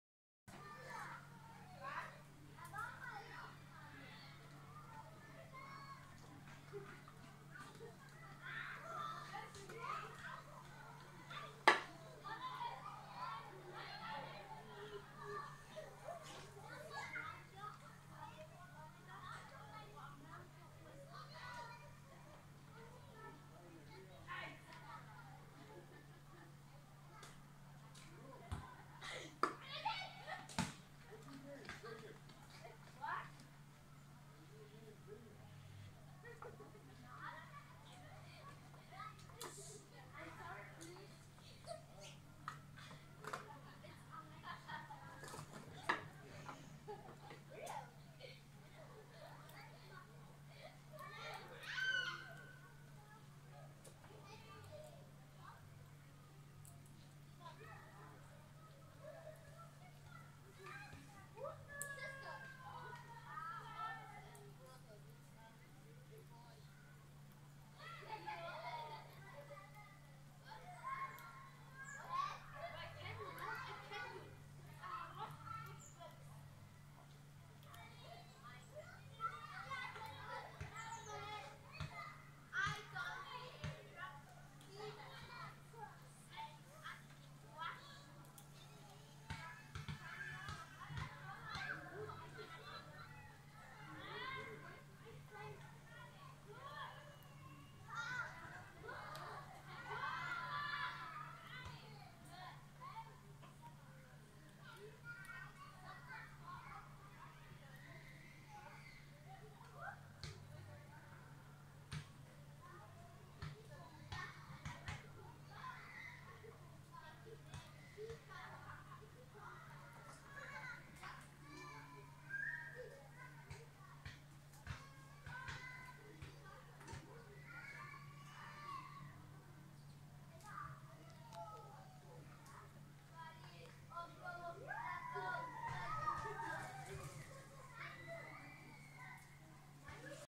Zhe Daily Business
Die Eichhörnchen tun, was sie jeden Tag tun - essen und neugierig sein